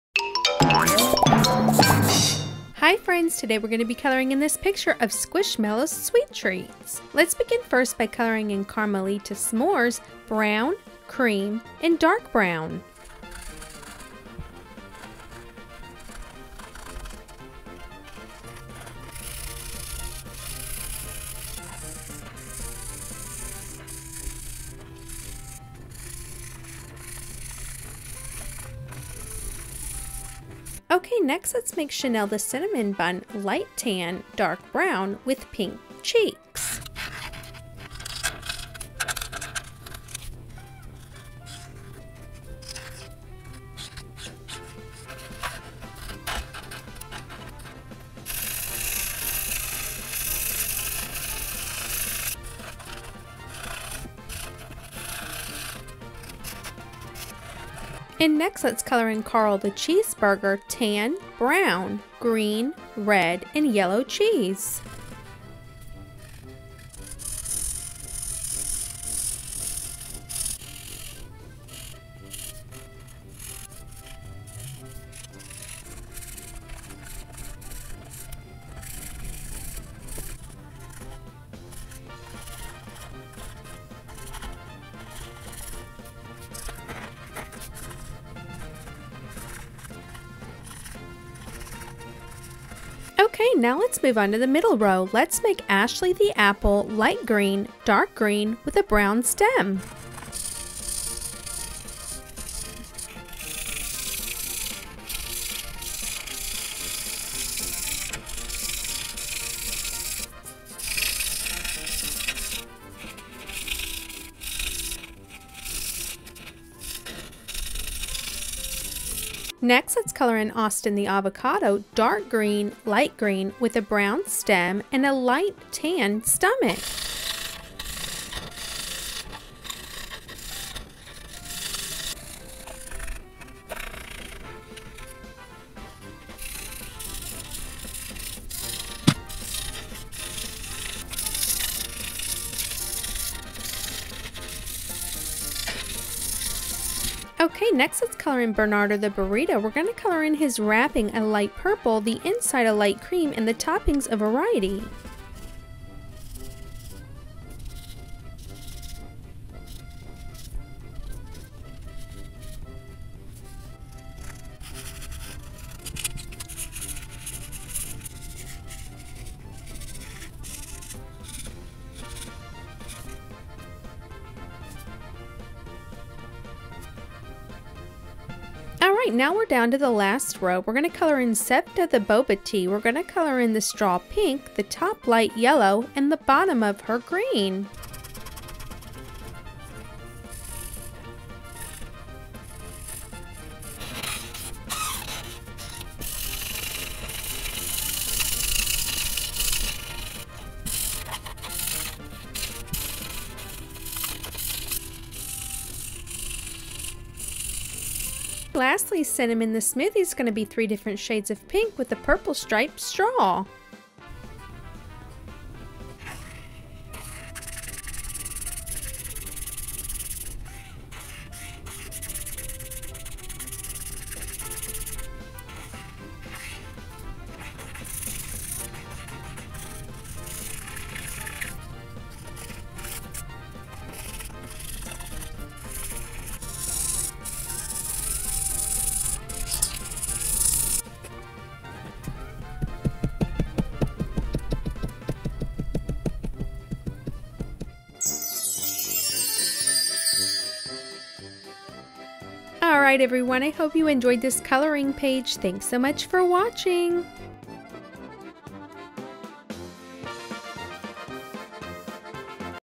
Coloring Squishmallows Sweet Treats Coloring Pages
Hi everyone,
In this video I am coloring a picture of squishmallows treats to eat and I had so much fun. If you love squishmallows, you will love this coloring page video.

Markers used:
Creative Expressions
Prismacolor
BIC
COPIC